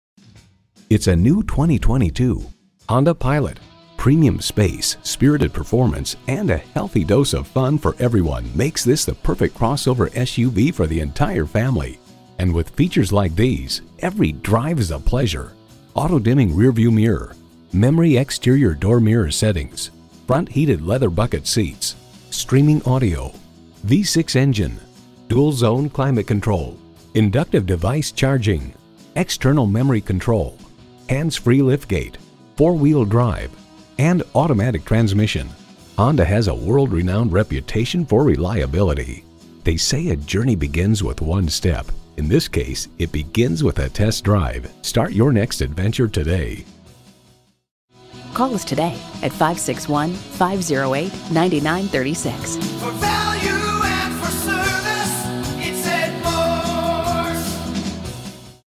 New 2022 Honda Pilot West Palm Beach Juno, FL NB089305 - SOLD
Year: 2022
Make: Honda
Model: Pilot
Trim: Special Edition
Engine: 3.5Ll 6 Cylinder Fuel Injected
Transmission: AWD 9-Speed Automatic
Interior: Black
Stock #: NB089305
VIN: 5FNYF6H2XNB089305
Crystal Black Pearl 2022 Honda Pilot Special Edition 3.5L V6 24V SOHC i-VTEC 9-Speed Automatic AWD 19/26 City/Highway MPG AWD.

Address:
3790 West Blue Heron Boulevard